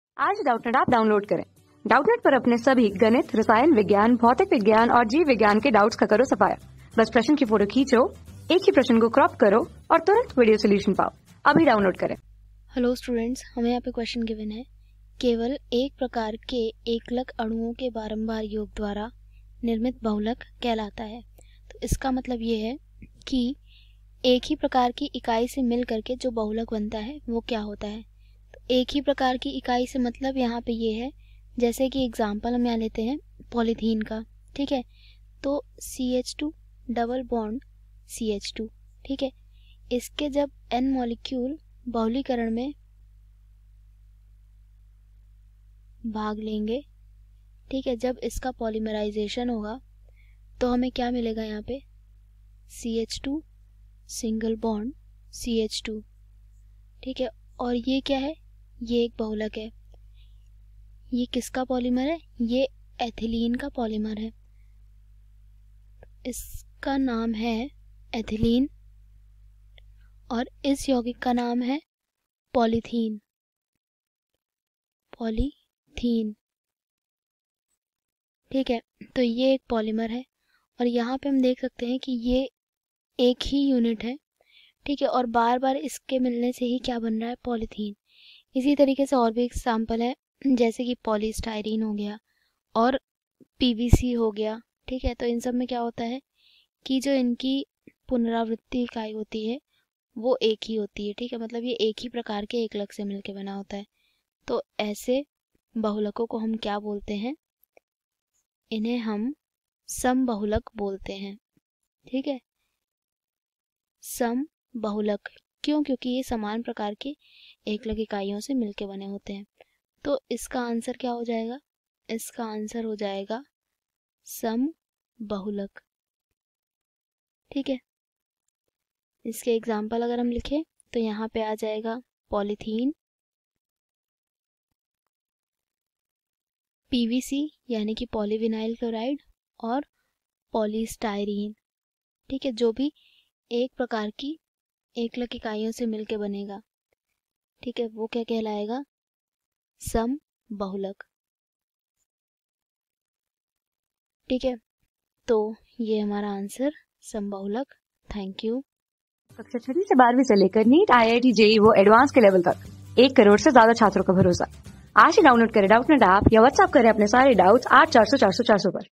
केवल एक प्रकार के एकलक अणुओं के बारम्बार योग द्वारा निर्मित बहुलक …………. कहलाता है । | 12 | बहुल...
केवल एक प्रकार के एकलक अणुओं के बारम्बार योग द्वारा निर्मित बहुलक …. कहलाता है ।
Class: 12
Subject: CHEMISTRY
Chapter: बहुलक
Board:UP Board

👉 Have Any Query? Ask Us.